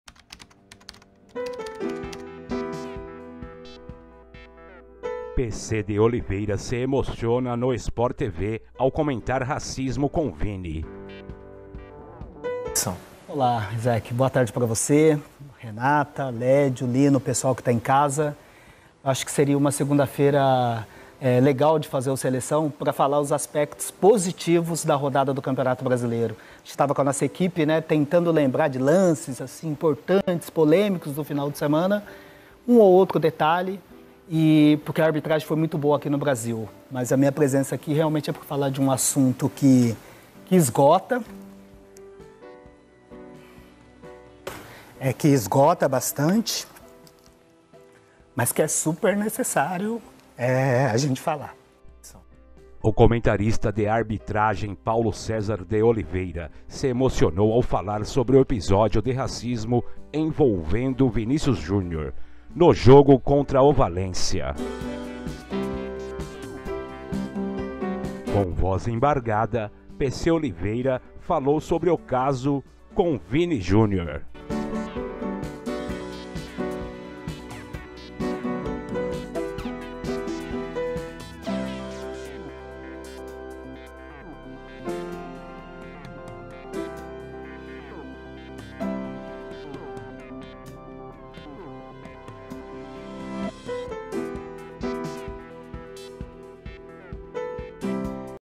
PC Oliveira se emociona ao comentar episódio com Vinicius Junior
PC de Oliveira se emociona no sportv ao comentar racismo com Vini: 'esgota' PC de Oliveira se emociona ao falar sobre caso de racismo com Vini Jr. Imagem: Reprodução/sportv Colaboração para o UOL, em Santos (SP) 22/05/2023 15h56 1 comentário CLASSIFICAÇÃO E JOGOS La Liga O comentarista de arbitragem Paulo César de Oliveira se emocionou ao falar sobre o episódio de racismo envolvendo Vinicius Jr. no jogo contra o Valencia. O que aconteceu